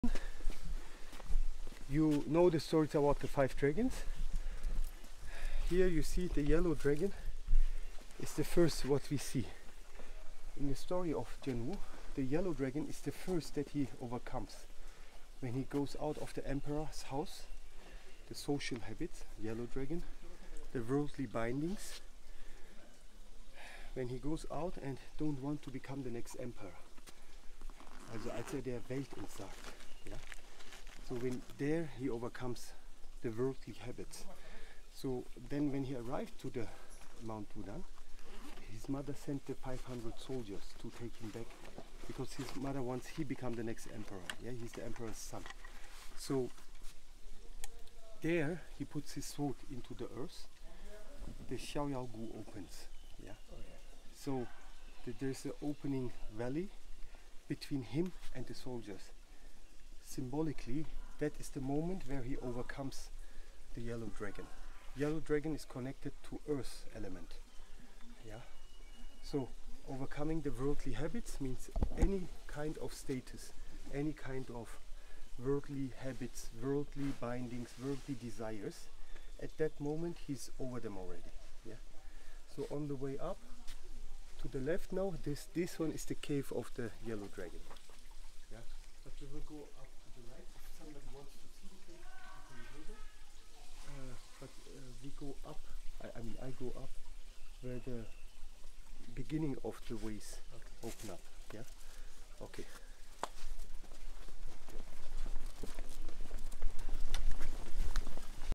Pt. 1: The Yellow Dragon - Another Way to discover the Wudang Mountains - with Ismet Himmet -
Ismet Himmet gives light on the meanings of the ancient symbols of the Mountain pilgrimage at Mt. Wudang.

In this episode we have reached the Yellow Dragon Cave, the place before the three heavenly gates on the YANG path up (DU MAI - Meridian) are divided from the YIN way down (REN MAI - Meridian on the Human body).

For more exclusive material look up: ismethimmetkungfu.com